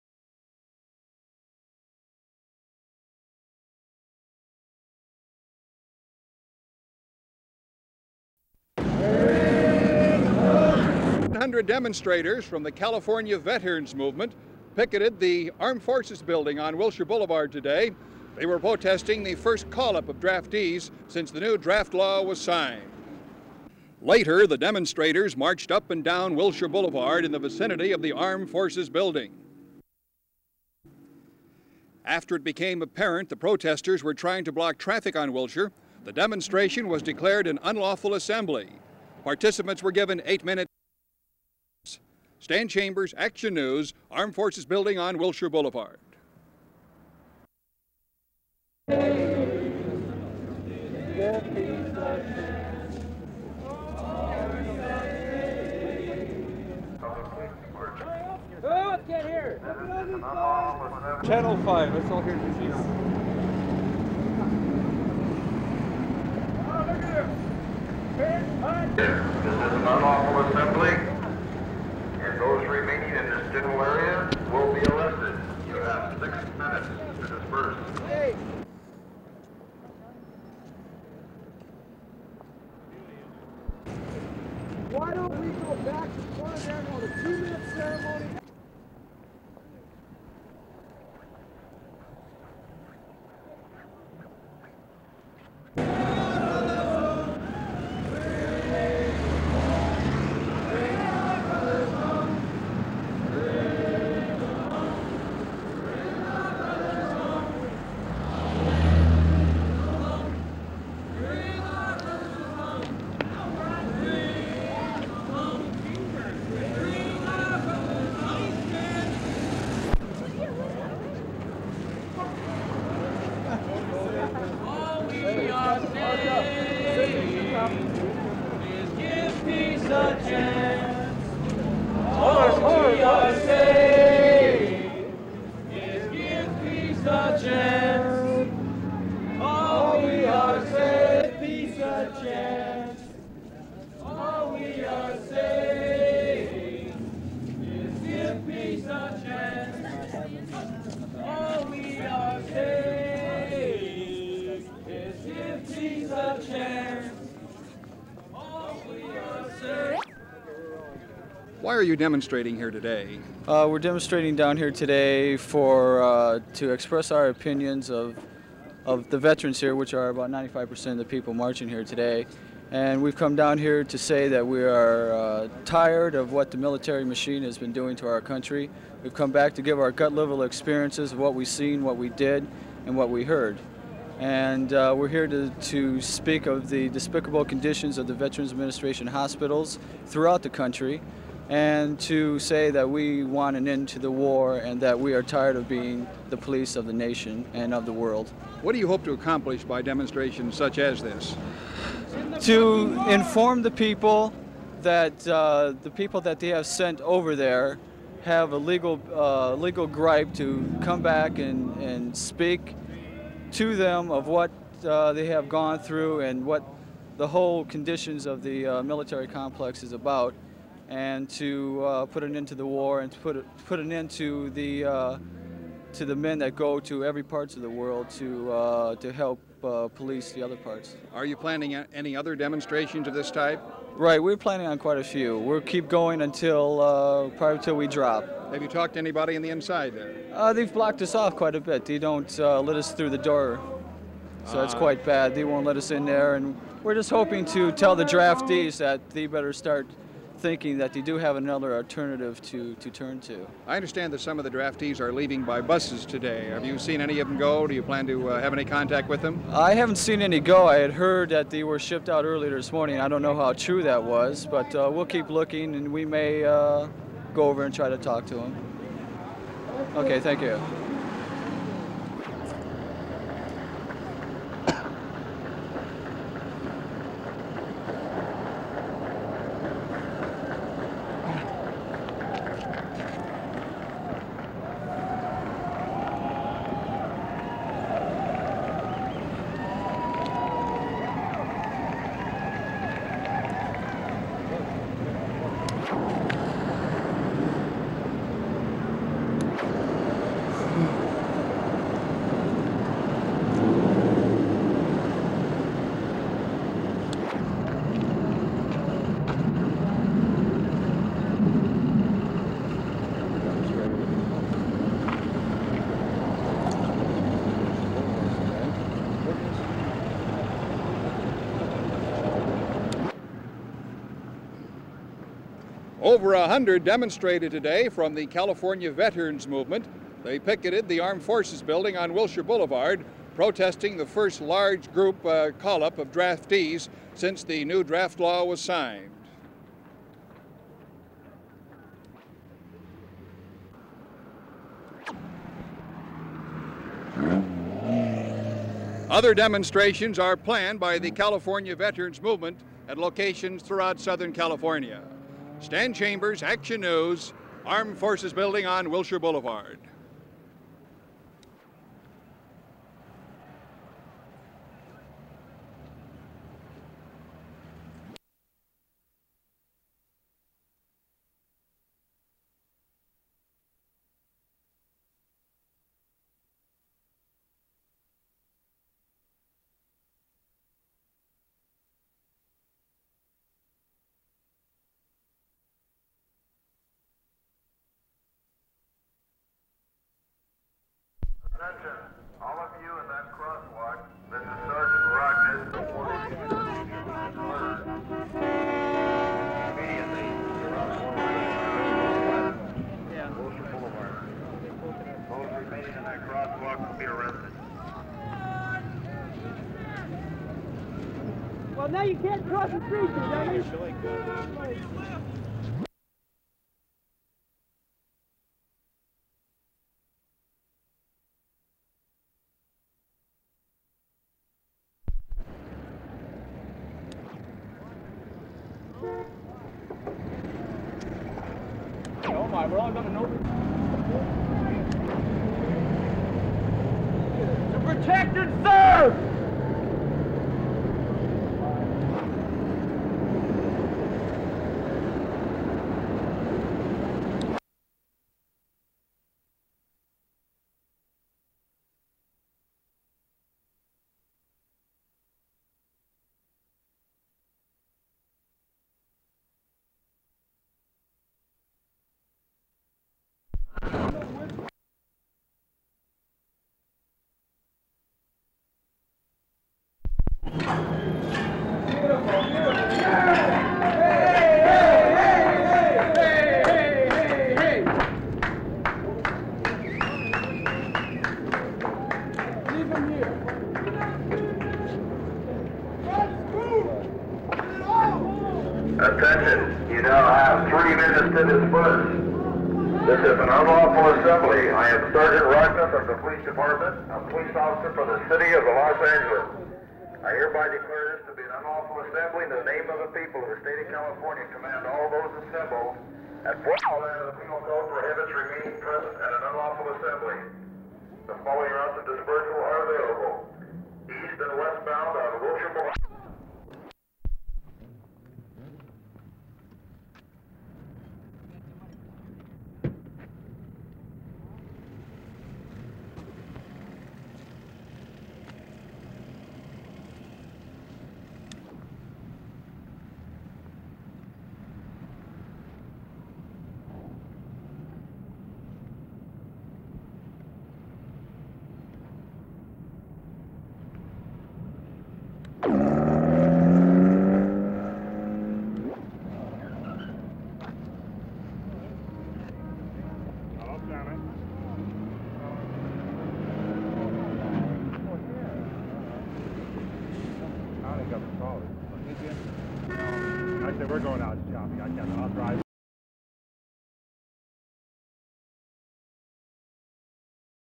KTLA News: "California Veterans Movement draft demonstration at Armed Forces Building" (1971)
The KTLA newsfilm collection at UCLA consists of cut and unedited stories, outtakes and fill footage, originally shot on 16mm reversal film stock with magnetic soundtrack. Some footage, particularly material not used for broadcast, may be without sound.

Title: "California Veterans Movement draft demonstration at Armed Forces Building." Date:  October 26, 1971.

Summary:  Demonstrators from the California Veterans Movement picketed in front of the Armed Forces Building in Los Angeles to protest the first call up of draftees since the new draft law was signed. At a later stage of the protest, demonstrators began blocking traffic causing the police to declare it an unlawful assembly and order them to disperse. Stan Chambers interviews one of the protesters about the reasons for the demonstration and what they hope to achieve. Footage includes shots of protesters carrying signs in front of the building, demonstrators walking in the streets, police preparing for action, and protesters walking away after the police give the order to disperse. There are also wide shots of the building, officers watching the protest from patrol cars, and demonstrators pouring red paint on the sidewalk and dipping their signs in the paint.

Learn more about this UCLA Film & Television Archive project: ucla.in/1SONCuw